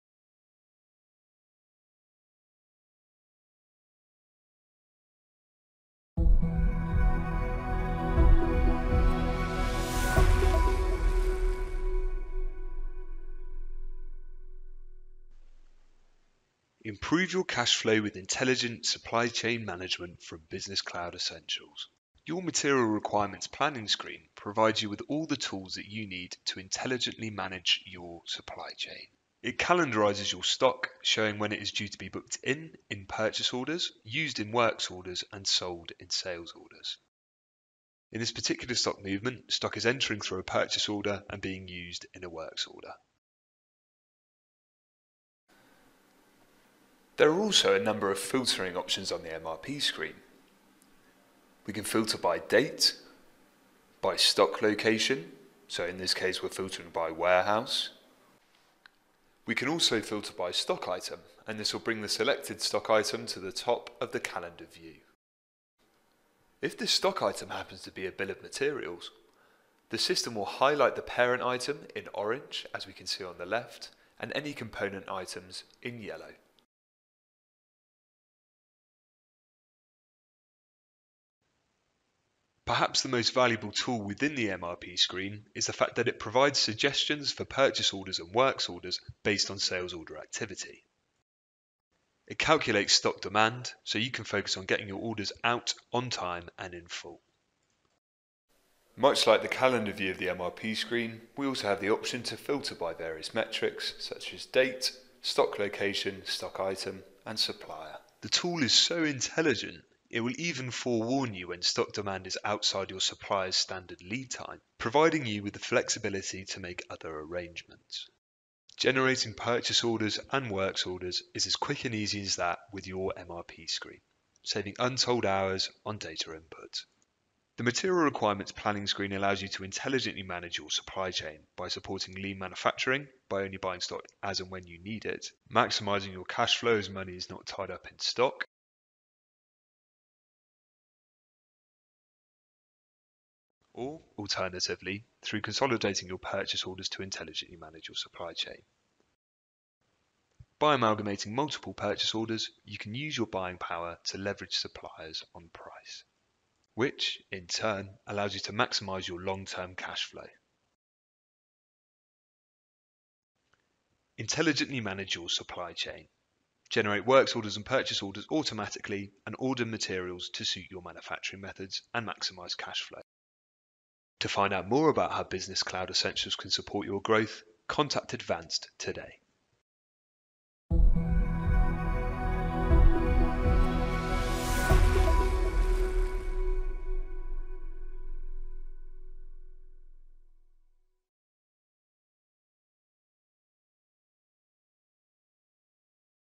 Improving Cashflow with Intelligent Supply Chain - Advanced Business Cloud Essentials Product Demo
A quick and easy way to understand the key features of Advanced Business Cloud Essentials.

How to improve cashflow with the intelligent supply chain management functionality.